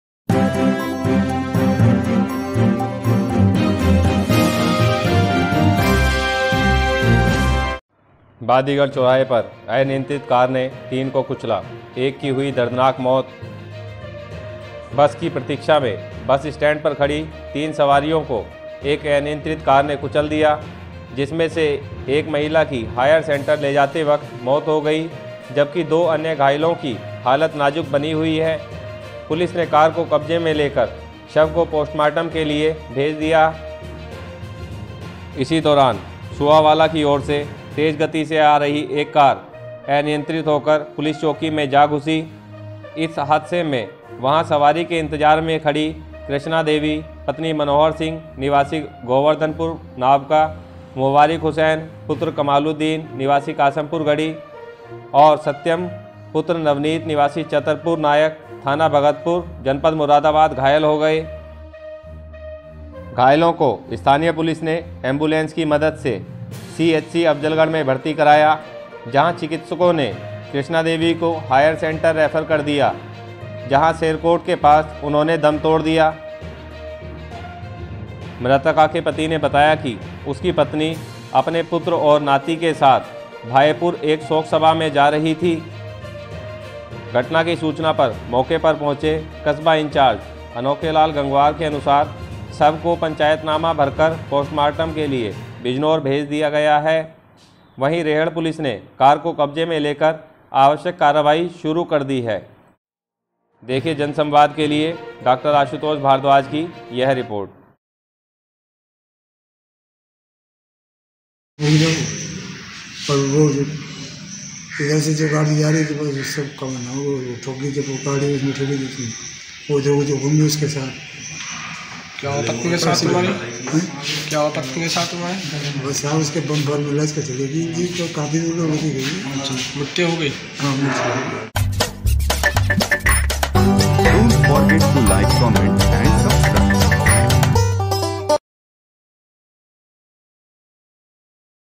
बादीगढ़ चौराहे पर अनियंत्रित कार ने तीन को कुचला, एक की मौत||Jansamvadnews
*बादी गढ़ चौराहे पर अनियंत्रित कार ने तीन को कुचला, एक की मौत।*

बस की प्रतीक्षा में बस स्टैंड पर खड़ी तीन सवारियों को अनियंत्रित कार ने कुचल दिया। जिसमें एक महिला की हायर सेंटर ले जाते हुए मौत हो गयी। जबकि दो अन्य घायलों की हालत नाजुक बनी हुई हैं। पुलिस के कार को कब्जे में लेते हुए शव को पोस्टमार्टम के  लिए भेजा दिया।
प्राप्त जानकारी के अनुसार बुधवार की सुबह बादीगढ़ चौराहे पर स्थित पुलिस चौकी के सामने कुछ सवारी बस की इंतजार में खड़ी थी। इसी दौरान गाँव सुआवाला की ओर से तेज गति से आ रही एक कार अनियंत्रित होकर पुलिस चौकी में जा घुसी। इस हादसे में वहां सवारी के इंतजार में खड़े कृष्णा देवी(60वर्ष) पत्नी मनोहर सिंह निवासी गाँव गोवर्धनपुर उर्फ़ नावका, मुबारिक हुसैन(56वर्ष) पुत्र कमालुद्दीन निवासी गाँव कासमपुरगढ़ी कोतवाली अफ़जलगढ़ व सत्यम कुमार (10वर्ष) पुत्र नवनीत निवासी गाँव चतरपुर नायक थाना भगतपुर(मुरादाबाद) घायल हो गयें।
घायलों को स्थानीय पुलिस ने एम्बुलेंस की मदद से सीएचसी अफ़जलगढ़ में भर्ती कराया। जहाँ चिकित्सको ने कृष्णा देवी को हायर सेंटर रेफर कर दिया।जहां शेरकोट के पास उन्होंने दम तोड़ दिया।
 मृतका के पति ने बताया कि वह कृष्णा देवी अपने नाती सत्यम व पुत्र धर्मेन्द्र के साथ भायपुर( ठाकुरद्वारा) एक शोक सभा मे जा रहे थें।
सूचना पर मौके पर पहुचें कस्बा इंचार्ज अफजलगढ़ अनोखेलाल गंगवार के अनुसार शव को पंचनामा  भरकर पोस्टमार्टम के लिए बिजनौर भेज दिया है। वहीं रेहड़ पुलिस ने कार को कब्जे में लेकर आवश्यक कार्रवाई की जा रहीं हैं।